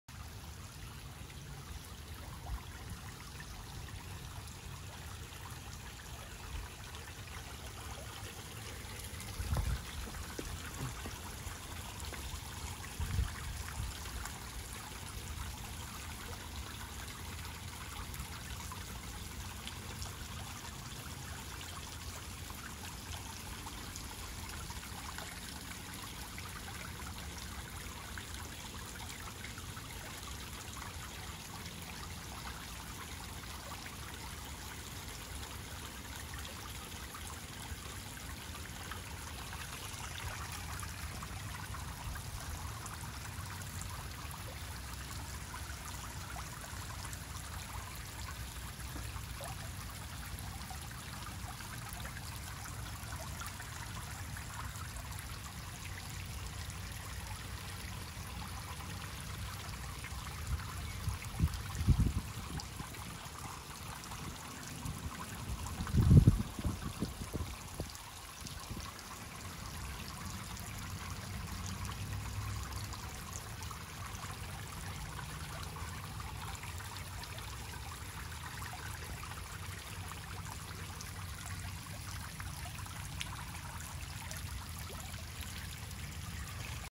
Backyard oasis
Sure the pond has been nice. We love hearing the water from the fountain and other water features. However the best parts have been adding the fish and plants. They bring so much life to this space in our backyard. We love it!